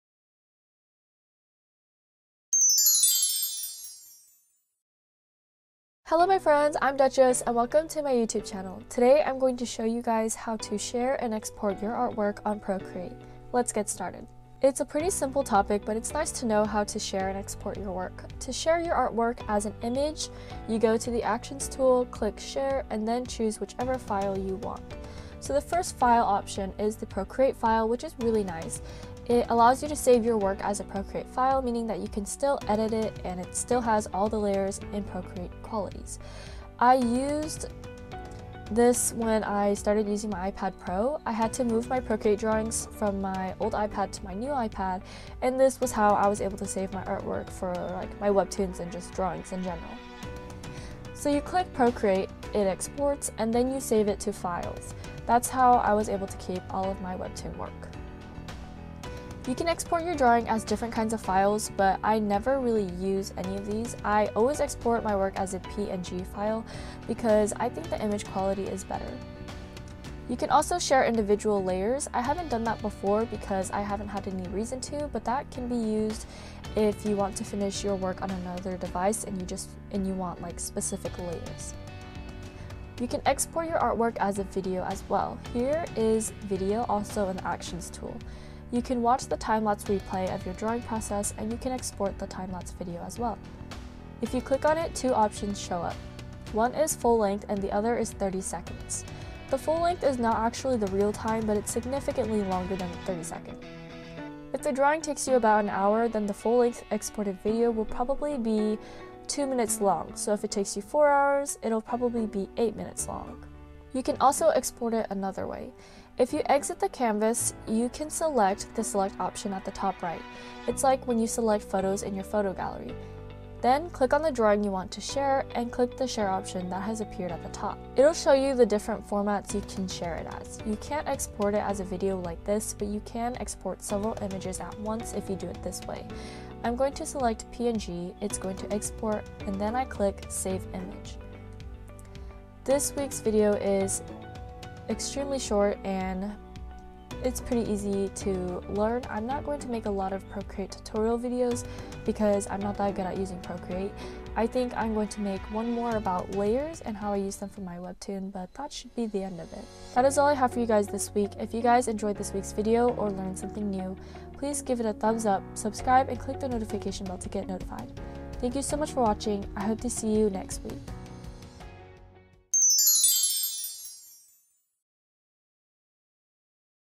How to Share your Artwork with PROCREATE
Here is the promised Procreate tutorial on how to export and share your artwork with procreate.

It's a very short video but helpful nonetheless. I hope you're all having a splendid week.

Tools Used:
Movavi Video Suite
Movavi Video Editing
iPad Pro
Procreate Pencil
Procreate App
Canva
MOVO
Offeo